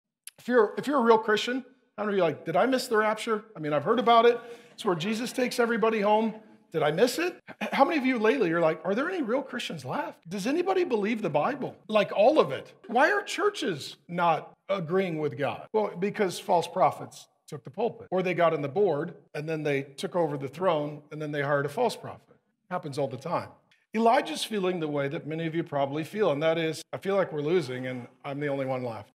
Did we miss the rapture?!
It may seem like it sometimes because of how dark the world has gotten. Are there any real Christians left? Do people actually believe the Bible?